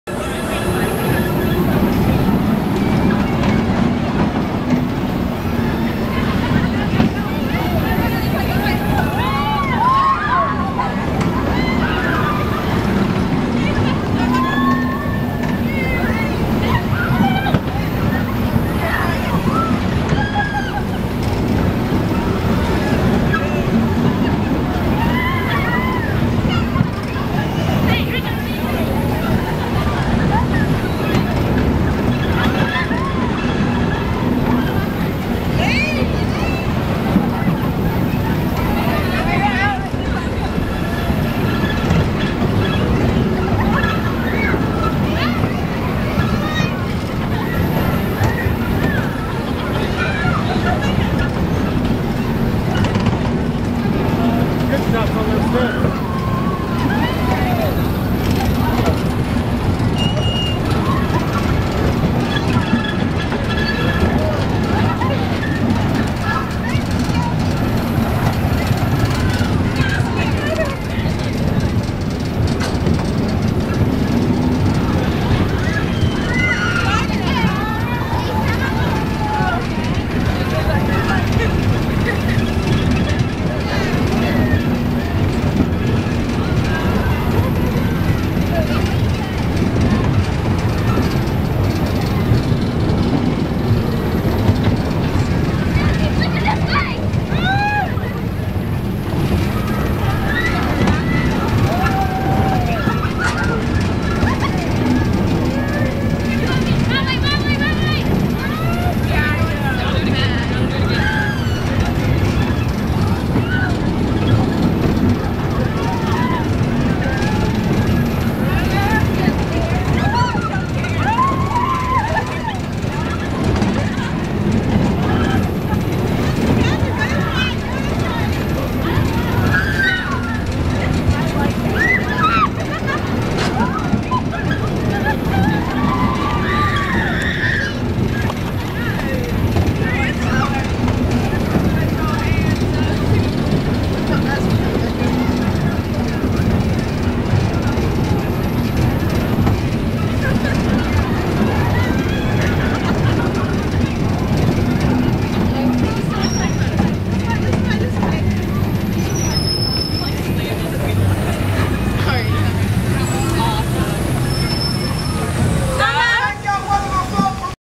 Tilt-a-Weevil
Kristin and Saylor having fun on the Tild-a-Weevil at Wild Adventures